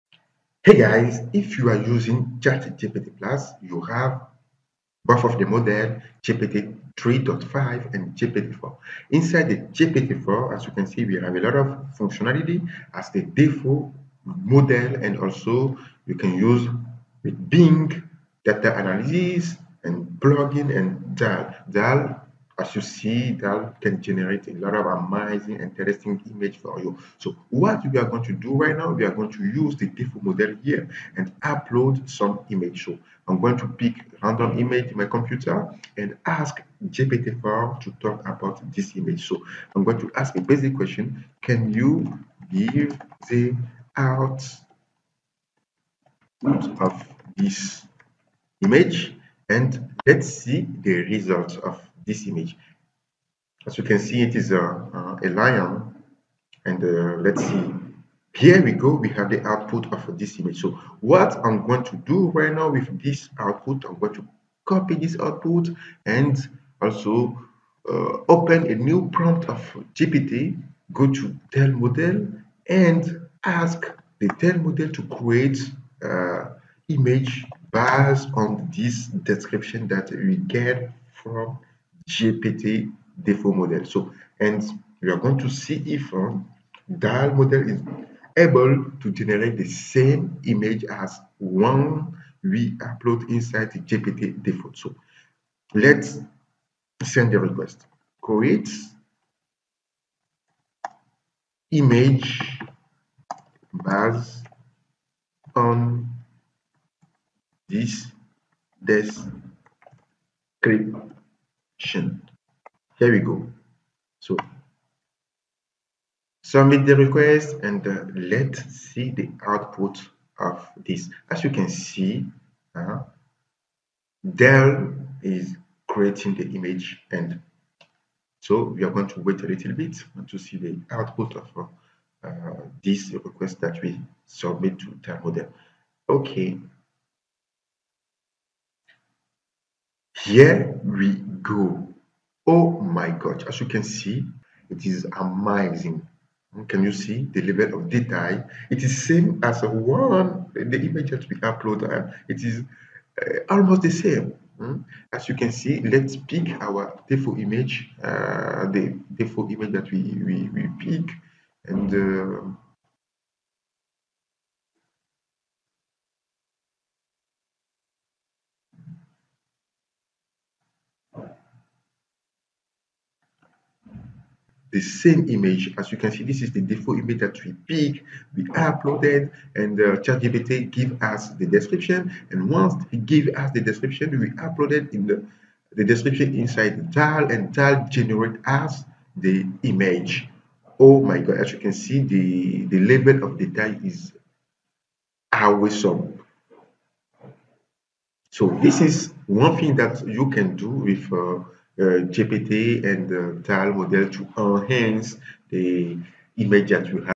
How to Generate and Analyze Images Using ChatGPT Plus and DALL-E
Explore Advanced AI Image Generation
In this video, I demonstrate how to leverage the powerful features of ChatGPT Plus, including GPT-3.5, GPT-4, and DALL-E, to generate and analyze images. Watch as I walk you through the process of uploading a random image, extracting its description, and using DALL-E to recreate the image with remarkable accuracy.

What You'll Learn:

Using ChatGPT Plus for Image Descriptions: Learn how to utilize the GPT models to generate detailed descriptions of images.
Image Creation with DALL-E: See how DALL-E can transform text descriptions into images, and compare the results with the original uploads.
Step-by-Step Guide: Follow along as I demonstrate how to recreate images and enhance them using AI tools.

Timestamps:

00:00 Introduction to ChatGPT Plus and DALL-E
00:30 Uploading and Describing Images with GPT-4
01:00 Creating Images with DALL-E
01:45 Comparing Original and Generated Images
02:30 Practical Uses of AI for Image Enhancement
03:20 Final Thoughts and Next Steps